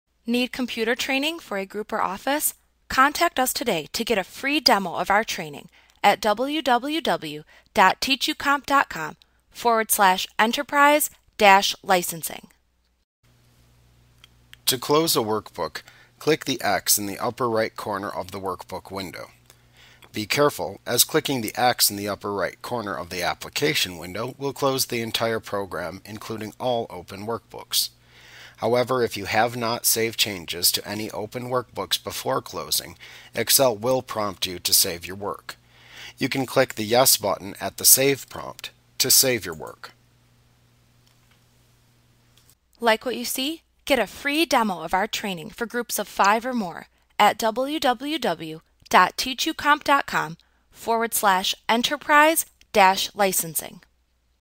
Microsoft Office Excel Tutorial 2013 Workbooks 2.8 Employee Group Training
A clip from Mastering Microsoft Excel Made Easy: close workbooks. Get a FREE demo of our training for groups of 5 or more